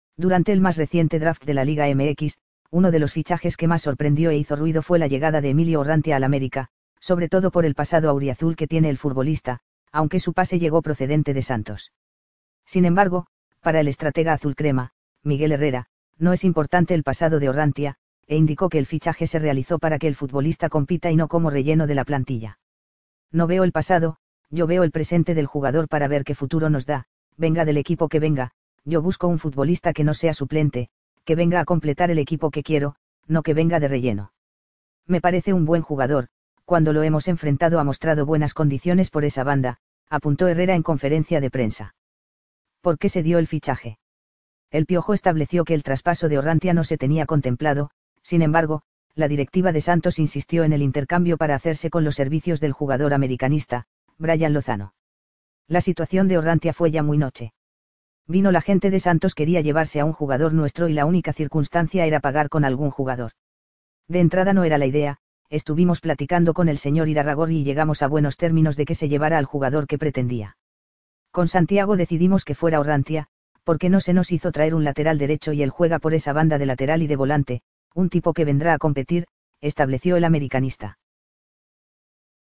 La Polemica Llegada de Orrantia al América!
Durante el más reciente Draft de la Liga MX, uno de los fichajes que más sorprendió e hizo ruido fue la llegada de Emilio Orrantia al América, sobre todo por el pasado auriazul que tiene el furbolista, aunque su pase llegó procedente de Santos

Sin embargo, para el estratega azulcrema, Miguel Herrera, no es importante el pasado de Orrantia, e indicó que el fichaje se realizó para que el futbolista compita y no como “relleno” de la plantilla.
“No veo el pasado, yo veo el presente del jugador para ver qué futuro nos da, venga del equipo que venga, yo busco un futbolista que no sea suplente, que venga a completar el equipo que quiero, no que venga de relleno. Me parece un buen jugador, cuando lo hemos enfrentado ha mostrado buenas condiciones por esa banda”, apuntó Herrera en conferencia de prensa.

¿Por qué se dio el fichaje?

El Piojo estableció que el traspaso de Orrantia no se tenía contemplado, sin embargo, la directiva de Santos insistió en el intercambio para hacerse con los servicios del jugador americanista, Brian Lozano.

“La situación de Orrantia fue ya muy noche. Vino la gente de Santos quería llevarse a un jugador nuestro y la única circunstancia era pagar con algún jugador. De entrada no era la idea, estuvimos platicando con el señor Irarragorri y llegamos a buenos términos de que se llevara al jugador que pretendía. Con Santiago decidimos que fuera Orrantia, porque no se nos hizo traer un lateral derecho y él juega por esa banda de lateral y de volante, un tipo que vendrá a competir”, estableció el americanista.